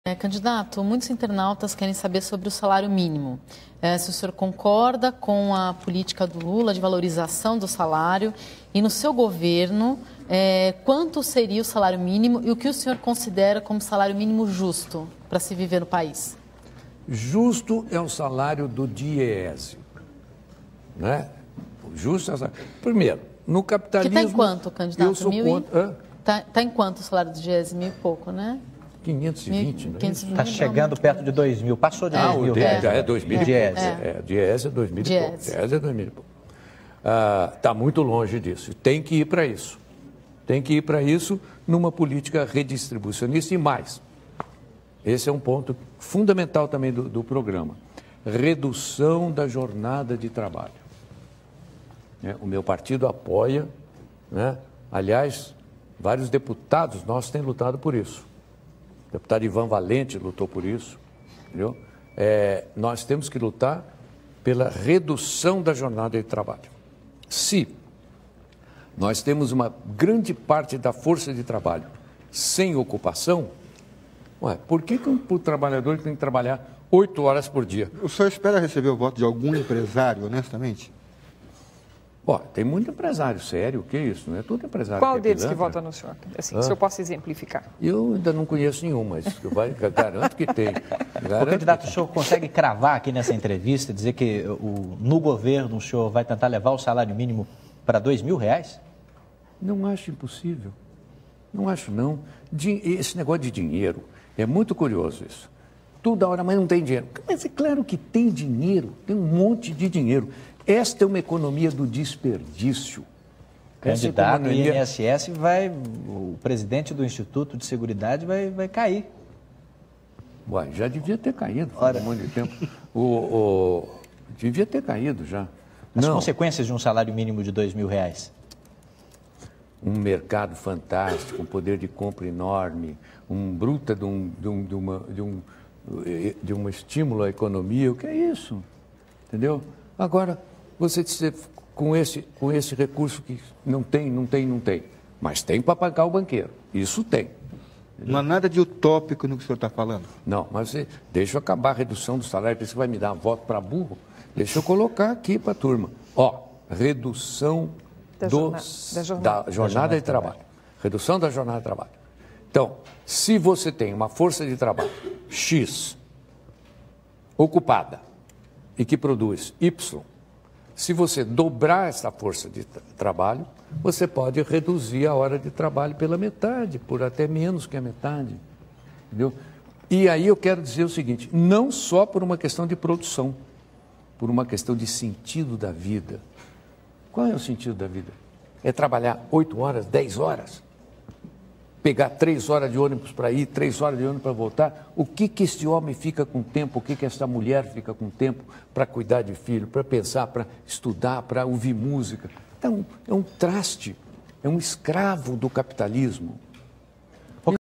movie 3
plinio arruda comenta sobre o salário mínimo.